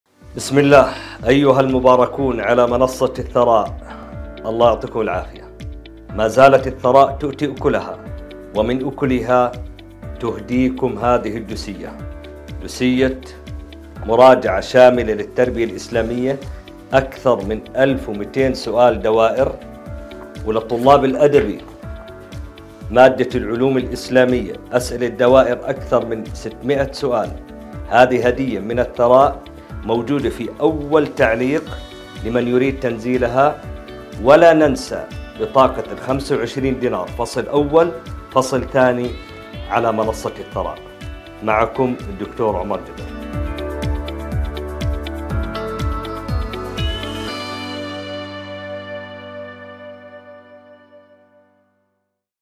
مراجعة شاملة للدكتور عمر جبر
ولأنكم مميزين حضرنالكم أسئلة مميزة في مادتي التربية_الإسلامية و العلوم_الإسلامية 🔥😍

مراجعة شاملة تم تحضيرها من الدكتور الفخم عمر جبر 🔥🔥💪🏻
ملفات المراجعة:
علوم إسلامية

لطلاب توجيهي_2005 🔥🤩